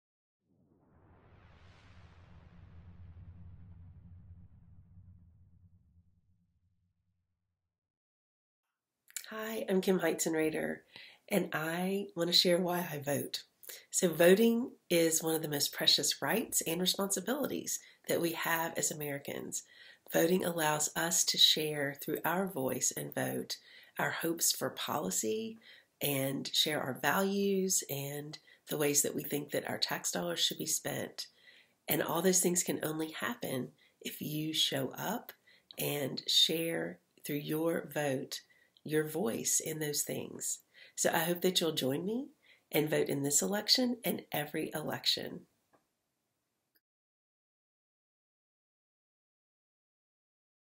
Kim Heitzenrater on WhyIVote
Today's video features Kim Heitzenrater,  the Director of the Career Center at Sewanee.

Kim Heitzenrater shares her response to WhyIVote

Presented by the Sewanee NAACP